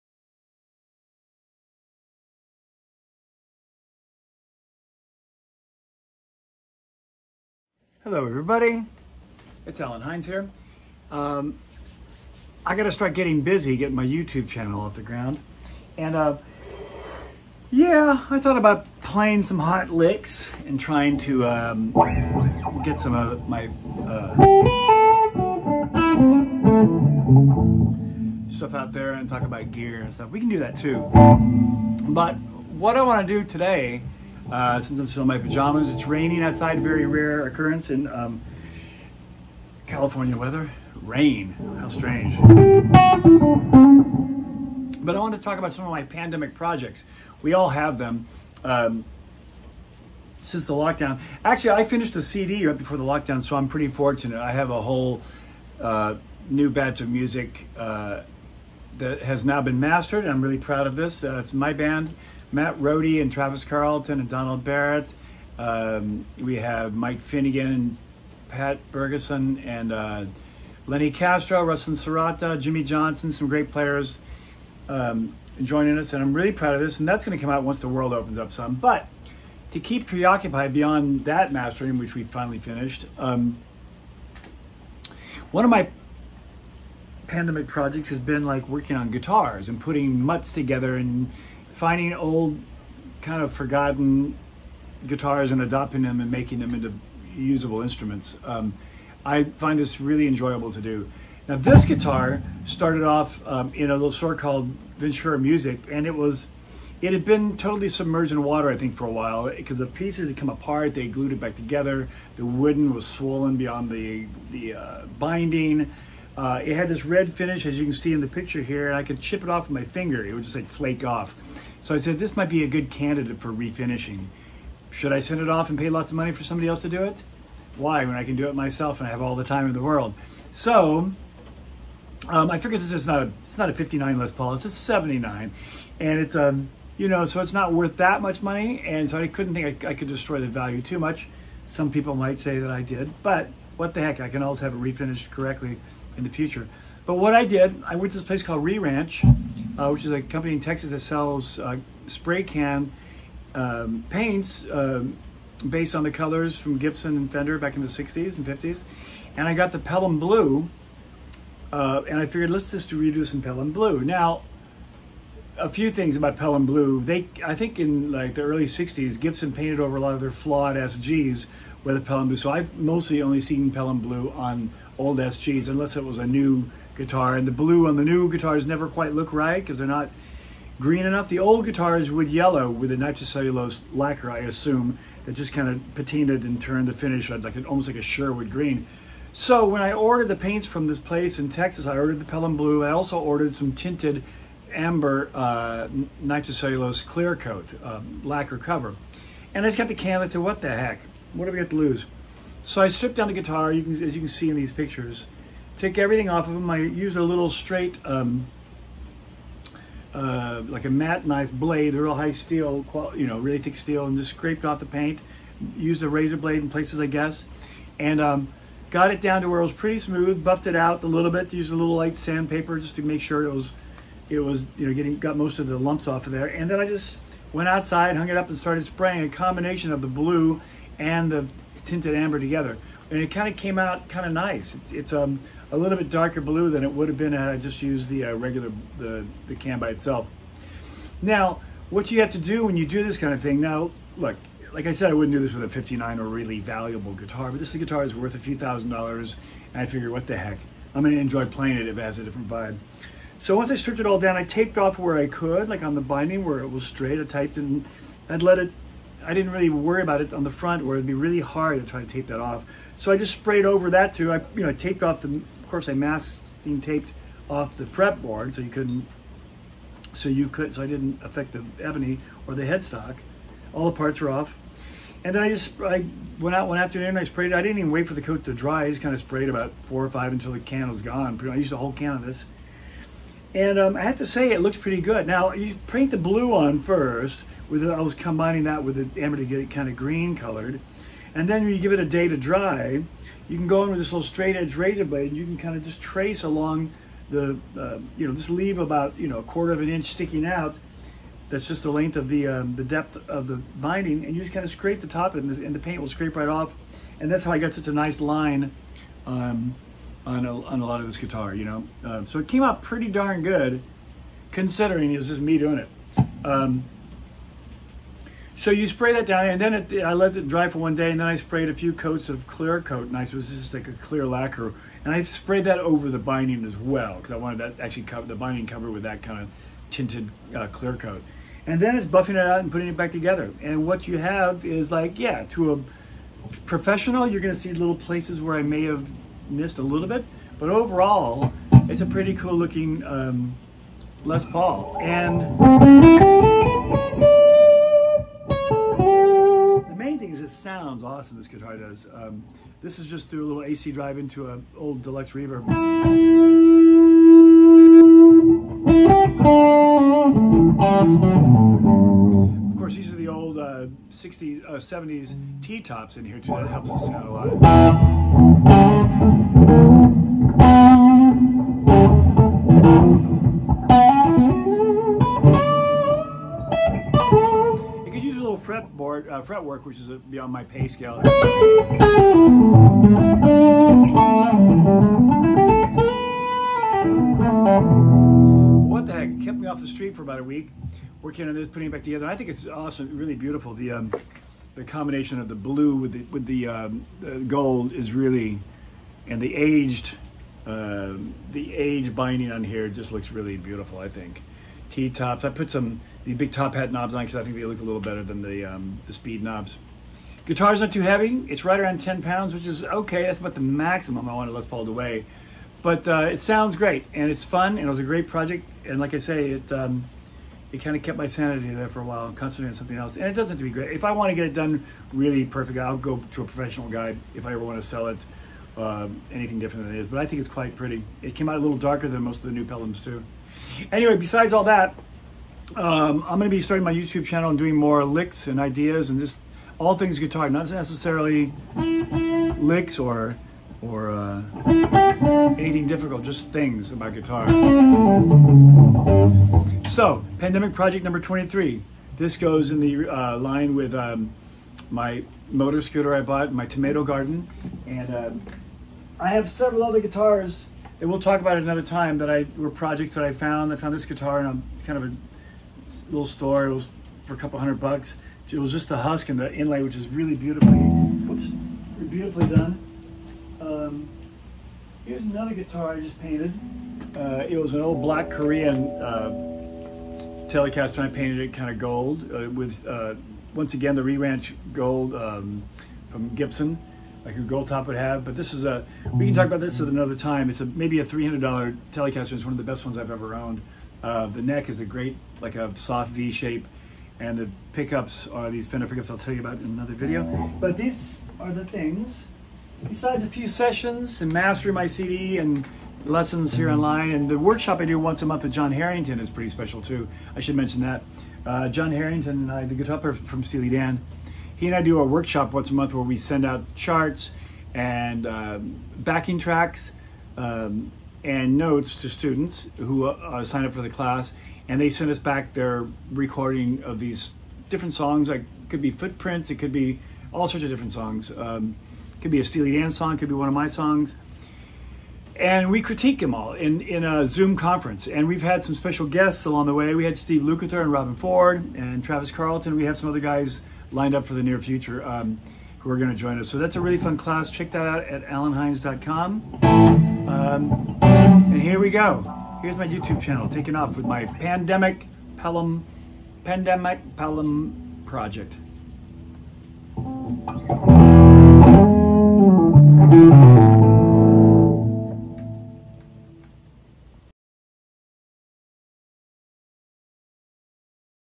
Pandemic Project #23
Too much time on my hands? well I found the nerve to paint an old Les Paul Custom myself...why not!!? heres how to do it yourself and a few other notes!